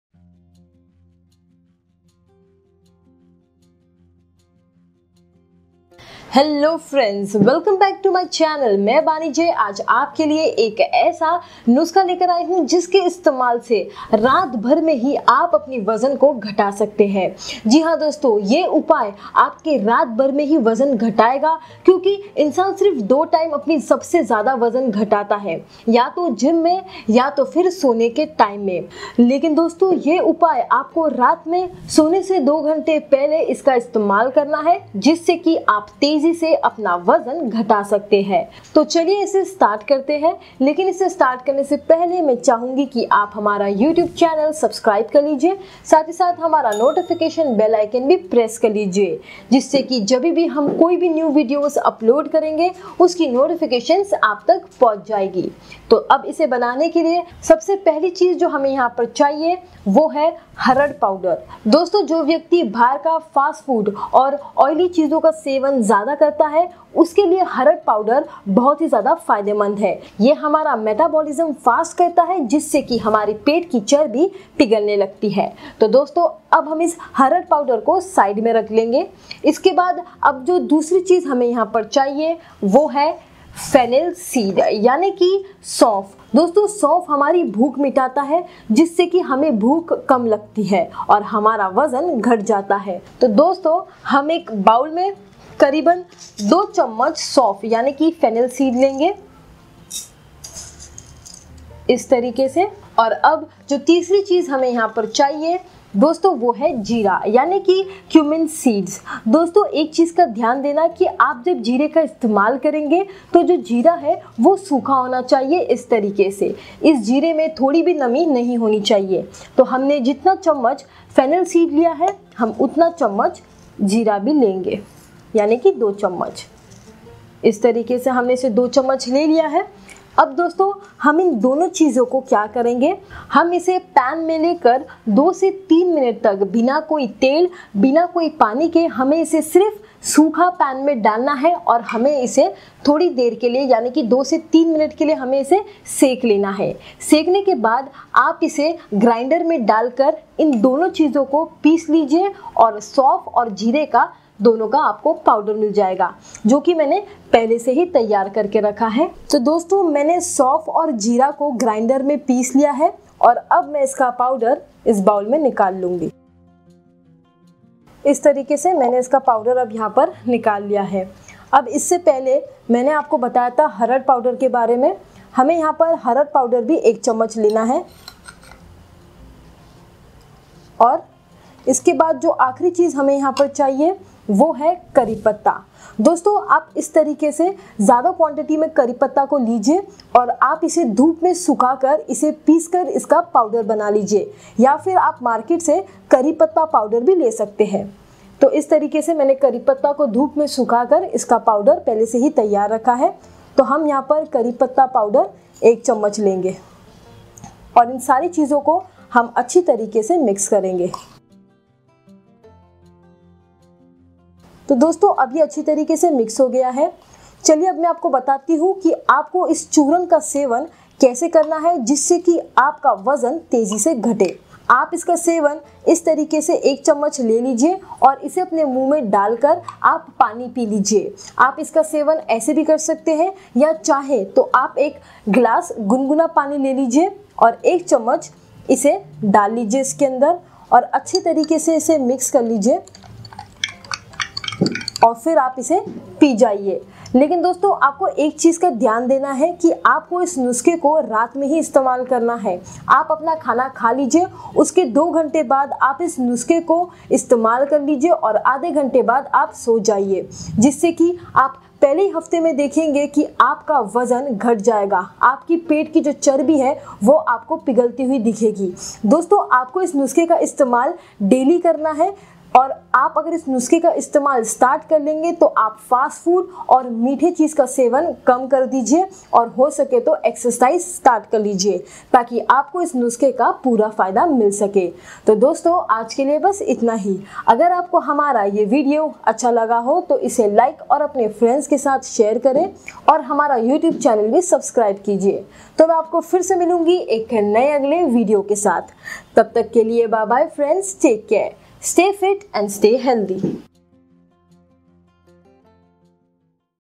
रात को 1 स्पून गुनगुने पानी के साथ पेट और कमर की चर्बी पसीना बनाकर बाहर निकल देगा weight lose remedy
रात को 1 स्पून गुनगुने पानी के साथ पेट और कमर की चर्बी पसीना बनाकर बाहर निकल देगा weight lose remedy || Rudra Home Remedies